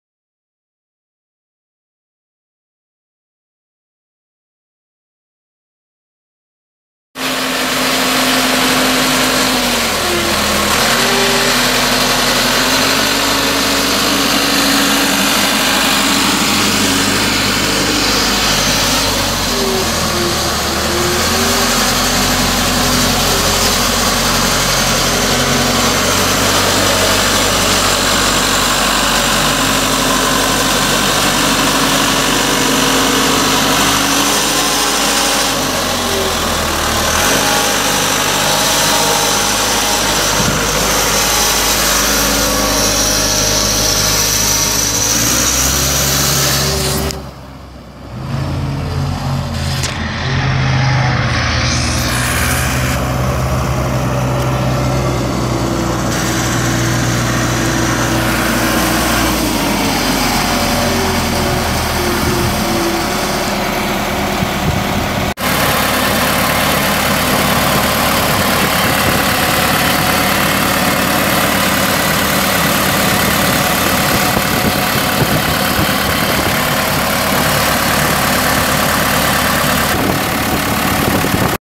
1997 Bomag BW11RH pneumatic roller for sale | no-reserve Internet auction March 17, 2016
1997 Bomag BW11RH pneumatic roller
937 hours on meter
Cummins 3.9L four cylinder diesel engine
Two speed hydrostatic transmission
OROPS model BW11RH
Front and rear water applicators
(9) tires
7.5-15 front tires
7.5-15LT rear tires
Serial A222C1546U